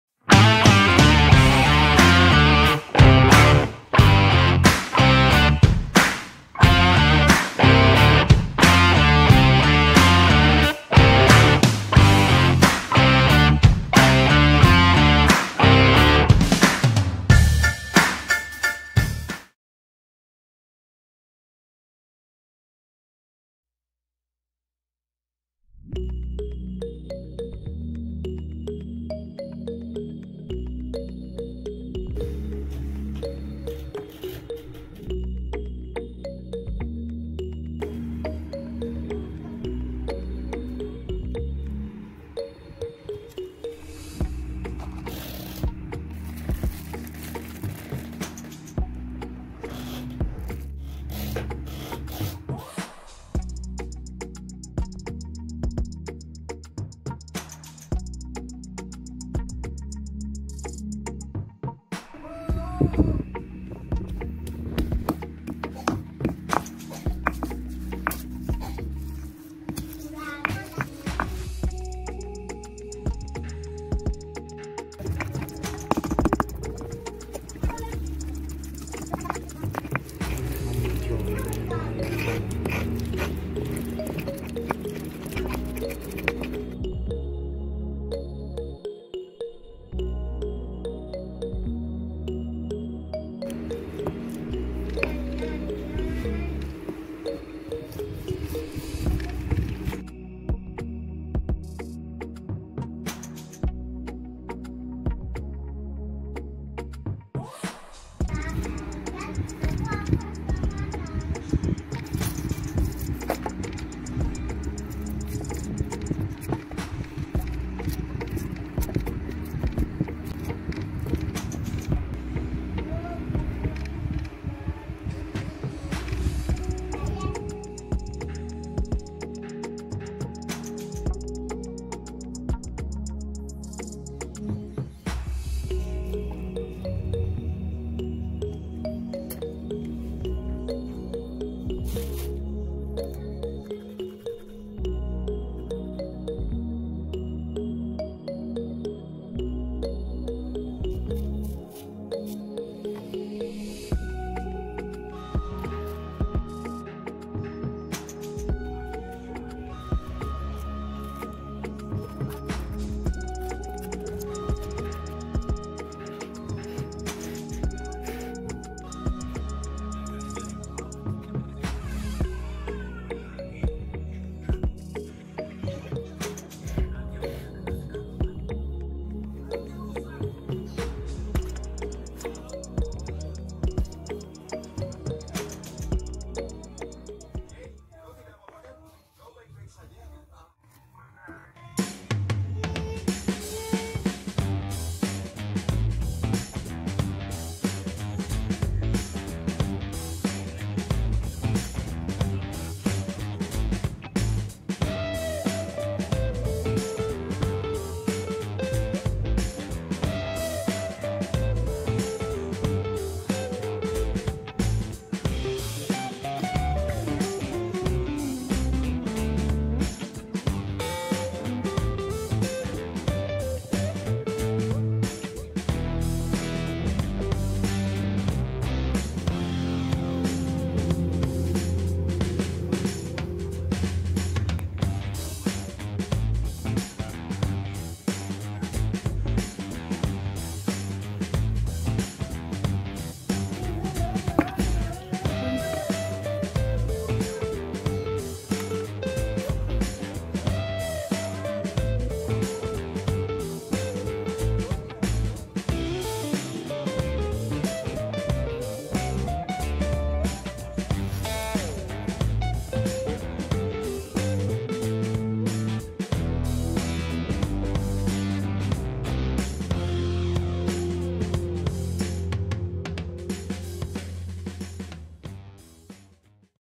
Cara Membuat Sambel Lotis Yang enak dan mudah !!
Cara membuat SAMBEL LOTIS bahan sederhana, Soal rasa Jangan ditanya ‼️ Asli MANTUL ‼️Streetfood-Java

Bahan-Bahan
▪︎Timun
▪︎Nanas
▪︎Bengkoang
▪︎Pepaya

Bahan sambal
▪︎Cabai 5 buah
▪︎Kencul 2cm
▪︎Gula Jawa 1 butir